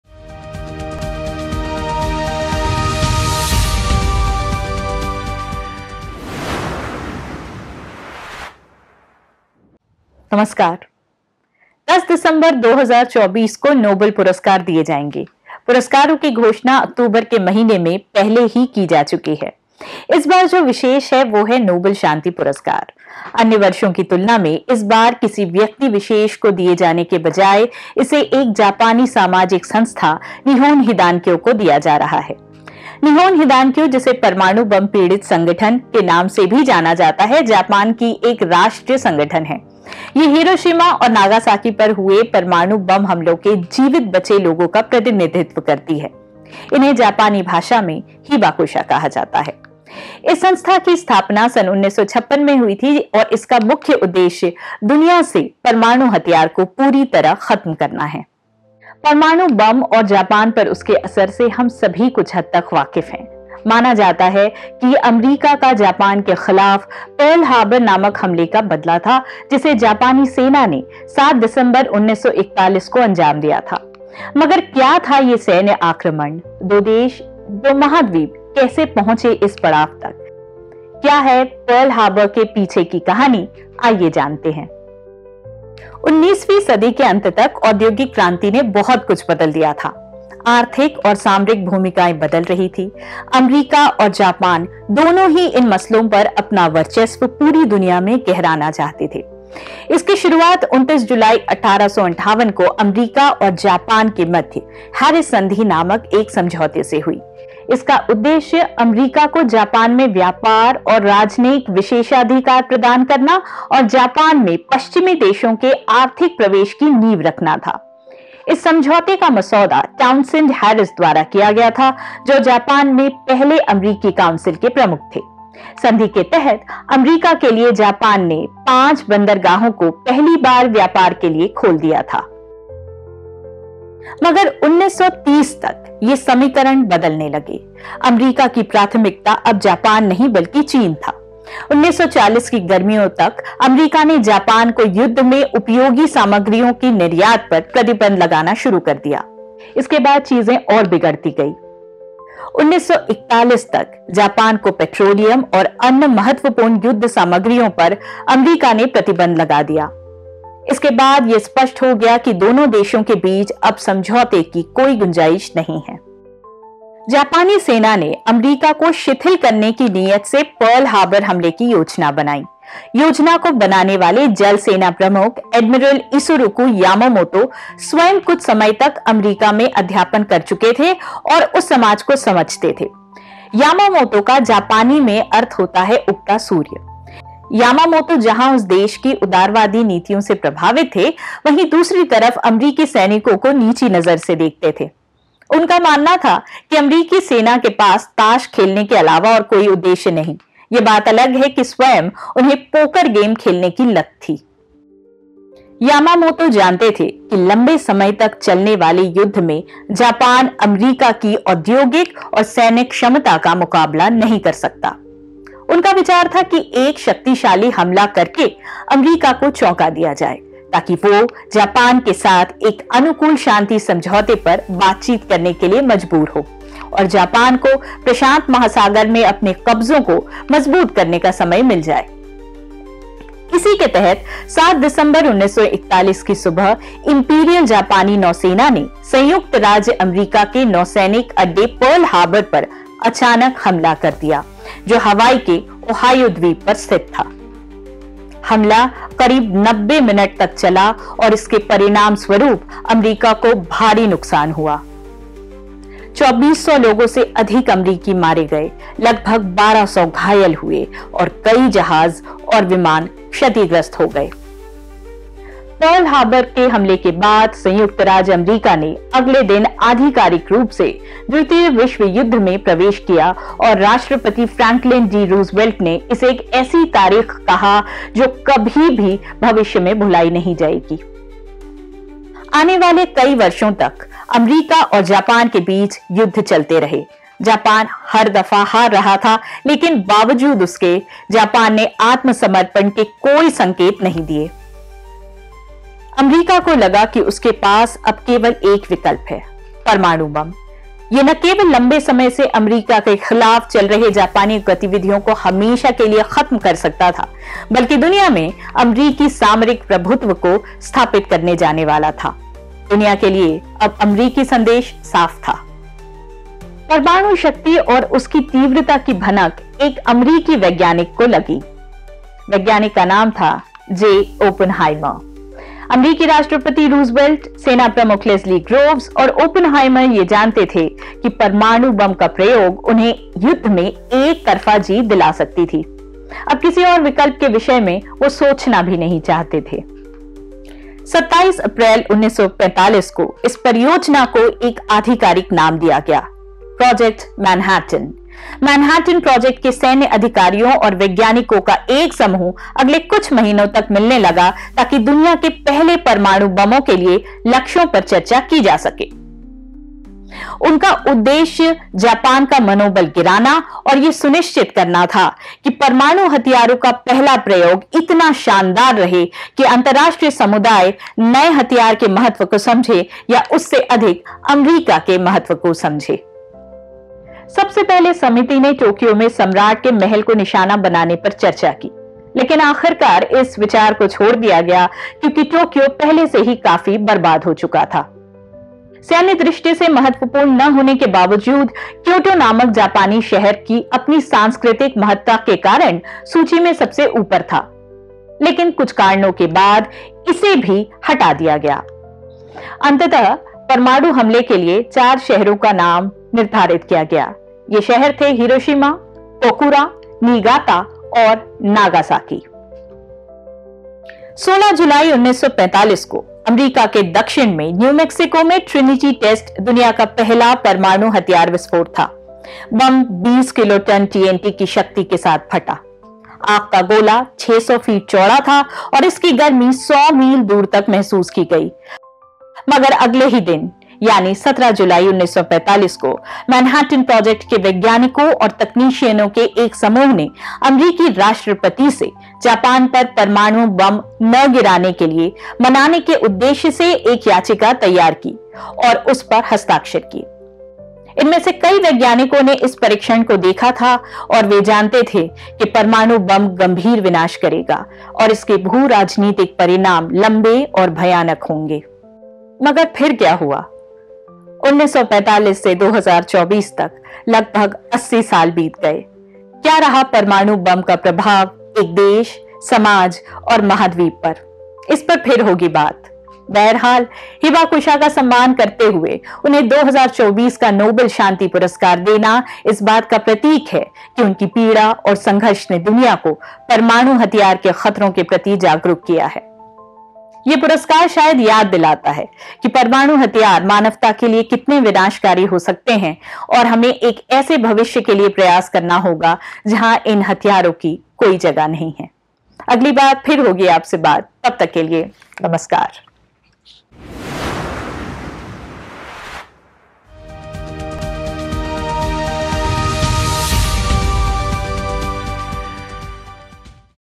पर्ल हार्बर : परमाणु हमले से नोबेल पुरस्कार तक...
10 दिसंबर 2024 को नोबेल पुरस्कार दिए जायेंगे. पुरस्कारों की घोषणा अक्टूबर के महीने में पहले ही की जा चुकी है। इस बार जो विशेष है वह है नोबेल शांति पुरस्कार. अन्य वर्षों की तुलना में इस बार किसी व्यक्ति विशेष के बजाय इसे एक जापानी सामाजिक संस्था निहोन हिदानक्यो को दिया जा रहा है. निहोन हिदानक्यो जिसे परमाणु बम पीड़ित संगठन महासंघ के नाम से भी जाना जाता है, जापान का एक राष्ट्रीय संगठन है. यह हिरोशिमा और नागासाकी पर हुए परमाणु बम हमलों के जीवित बचे लोगों का प्रतिनिधित्व करता है, जिन्हें हिबाकुशा कहा जाता है. इस संस्था की स्थापना 1956 में हुई थी, जिसका मुख्य उद्देश्य दुनिया से परमाणु हथियार को पूरी तरह ख़त्म करना है.

परमाणु बम, और जापान पर उसके असर से हम सभी कुछ हद तक वाकिफ हैं. माना जाता है कि ये अमरीका का जापान के खिलाफ़ पर्ल हार्बर नामक हमले का बदला था, जिसे जापानी सेना ने 7 दिसंबर 1941 को अंजाम दिया था. मगर क्या था सैन्य आक्रमण, दो देश, दो महाद्वीप कैसे पहुंचे यहाँ तक? क्या है इसके पीछे की कहानी, आइये जानते हैं.